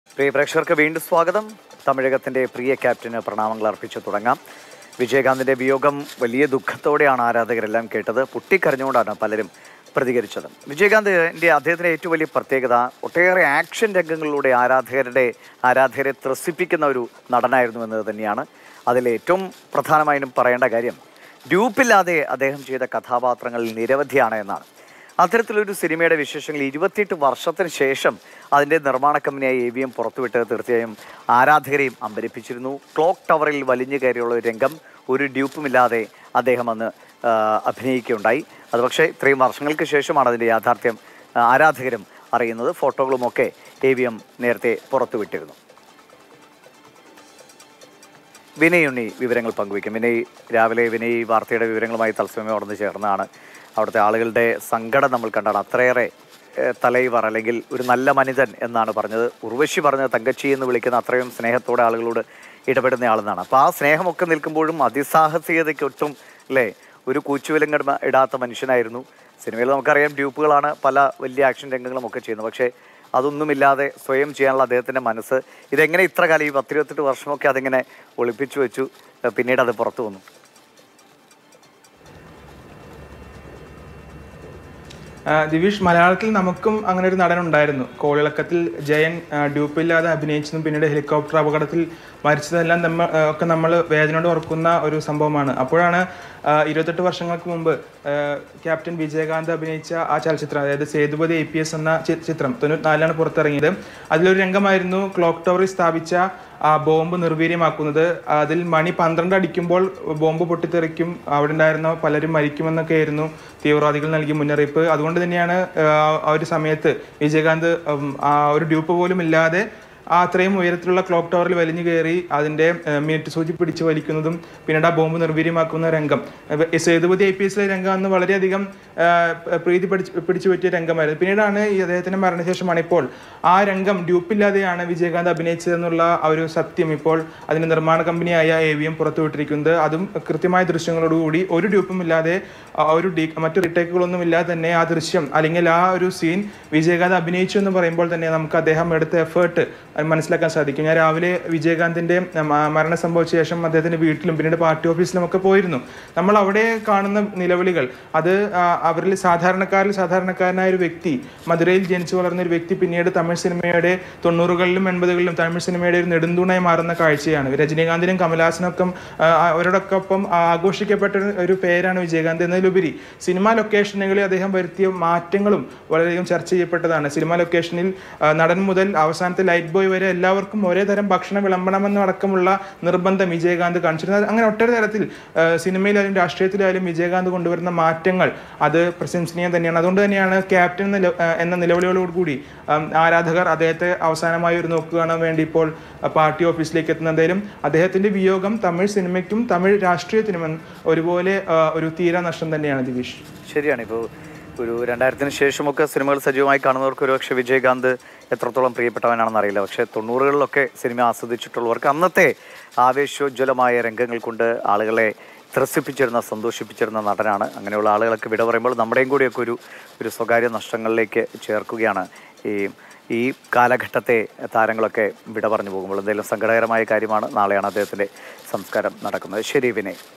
ഡ്യുപ്പ് ഇല്ലാതെ ക്ലോക്ക് ടവറിൽ! വിജയ് കാന്ത് ആരാധകരെ അമ്പരപ്പിച്ച രഹസ്യം പുറത്തുവിട്ട് AVM
Happy viewing!